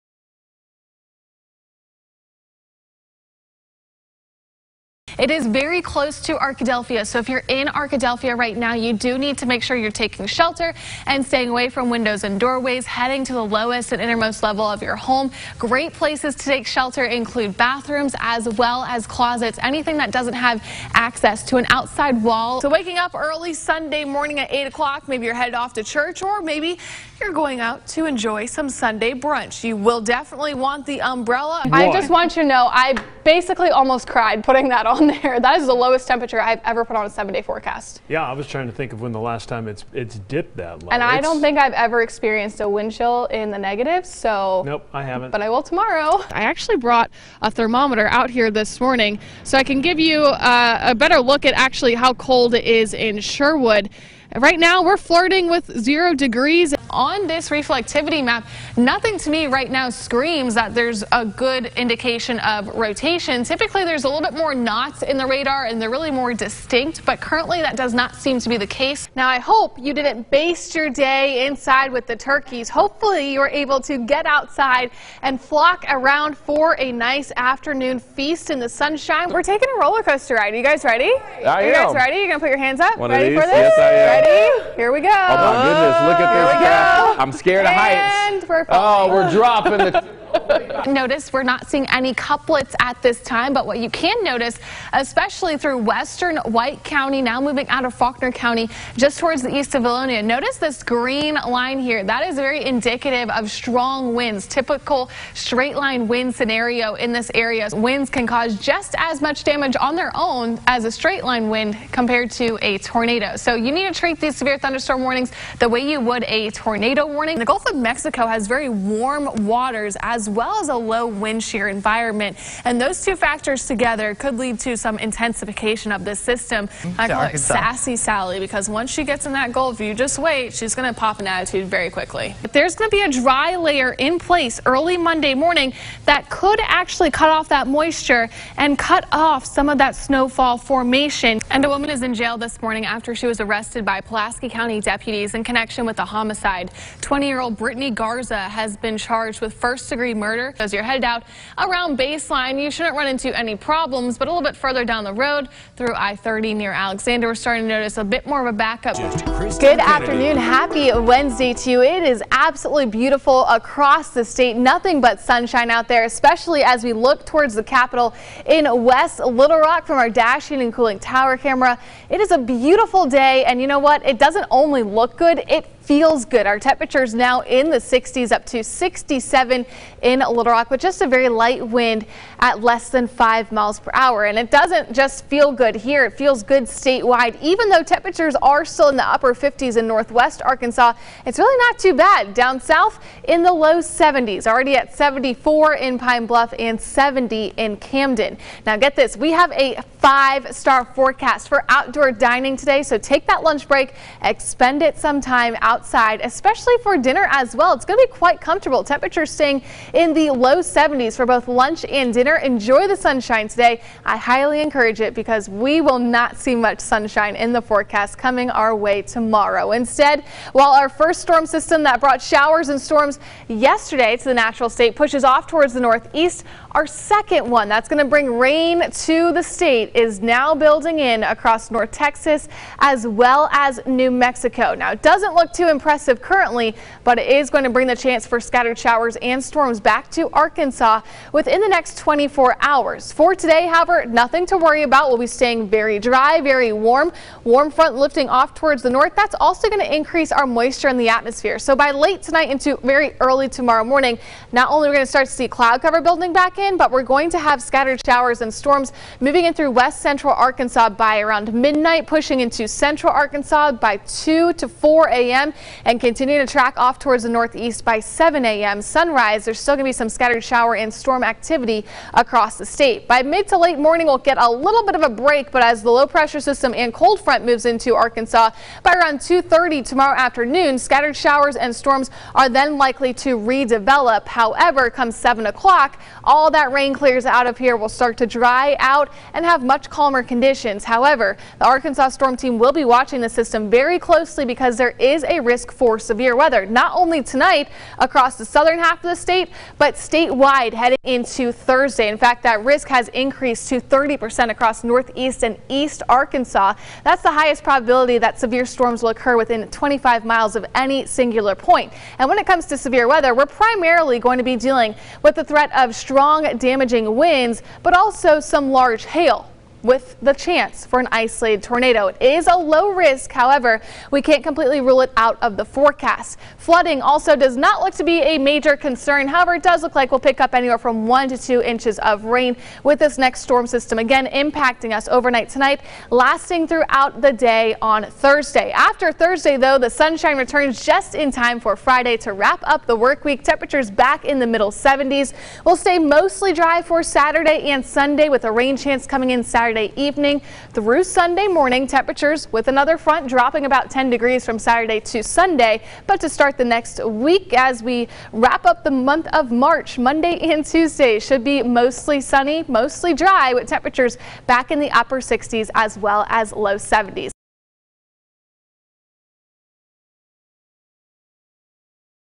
Meteorologist Kristen Kennedy ~ March 2021 Resume Reel
This reel was created in late March 2021 and includes some of my best work as a broadcast meteorologist. The first couple minutes are a montage of clips that include severe weather coverage, pops of personality, meteorological explanations, live shots, etc. There is also one full forecast located about 2-3 minutes into the video.